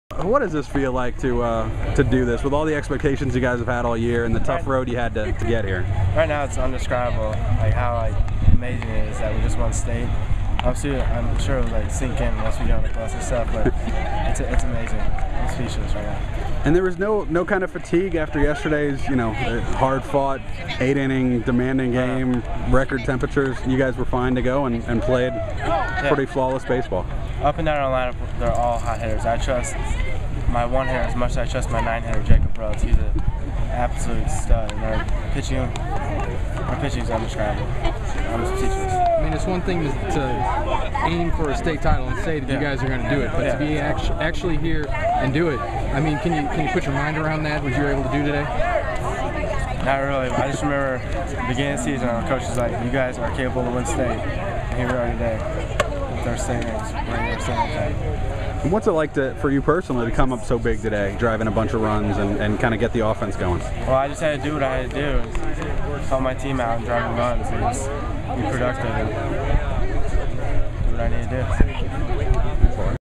Kyle Nuetzel 6-4-11
Francis Howell senior first baseman Kyle Nuetzel talks about his team's victory in the Missouri Class 4 championship game on June 4, 2011.